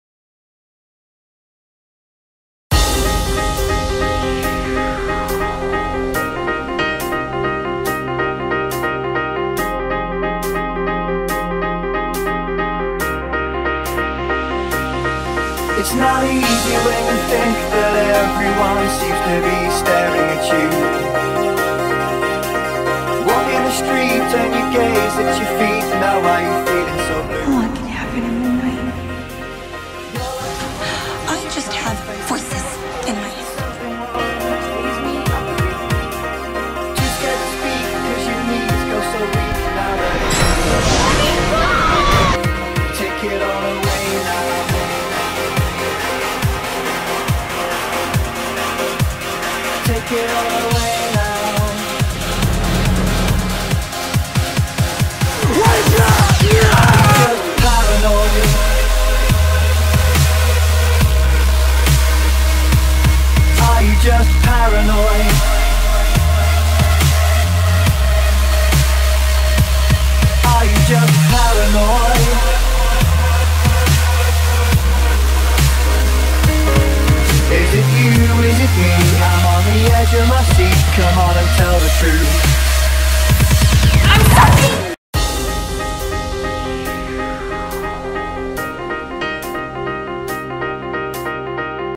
Lunatics Trailer
This is a short trailer for a book I'm writing on Wattpad. The link to the book is below. This is my first trailer so I hope it doesn't suck too badly because I enjoyed making it. :D